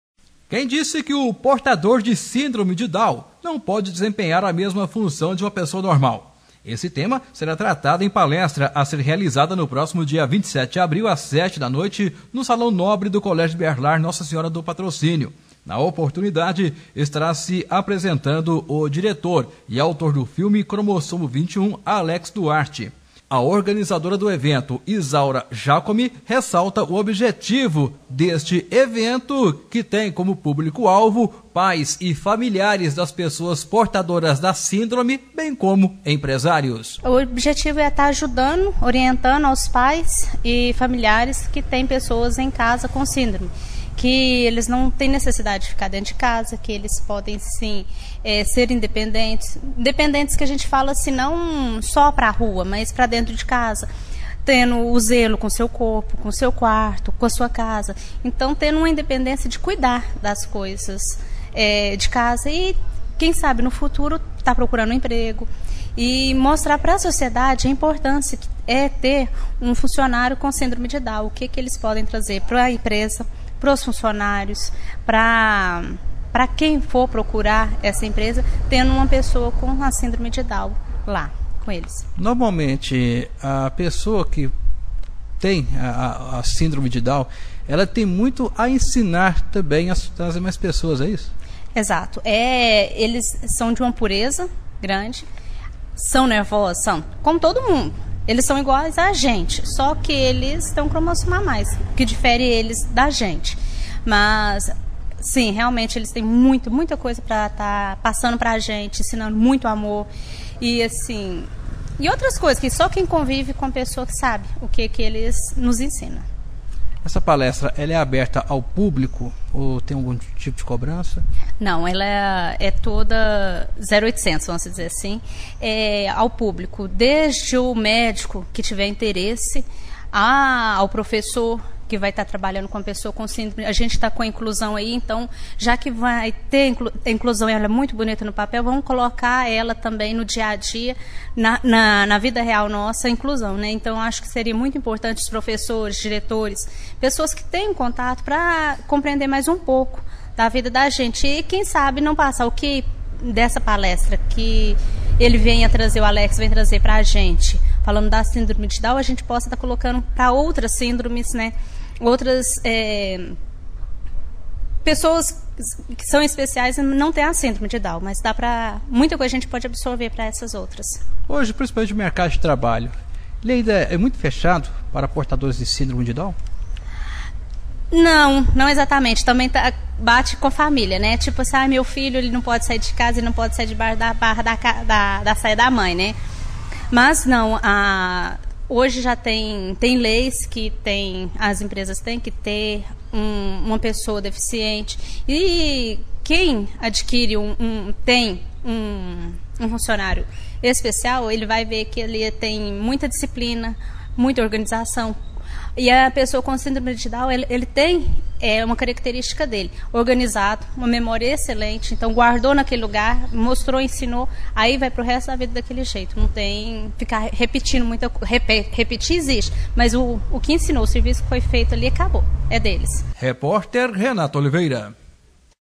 Palestra vai abordar Síndrome de Dow
A palestra será realizada no próximo dia 27 de abril, às 19h, no salão nobre do Colégio Berlaar Nossa Senhora do Patrocínio e terá Alex Duarte como palestrante.